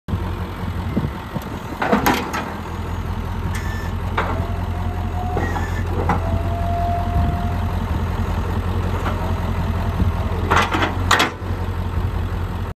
Western snow plow for sale at auction | bidding closes March 9, 2022
Western  snow plow
 Serial: B1054475
 Width: 8'6"
 Will be removed from truck prior to auction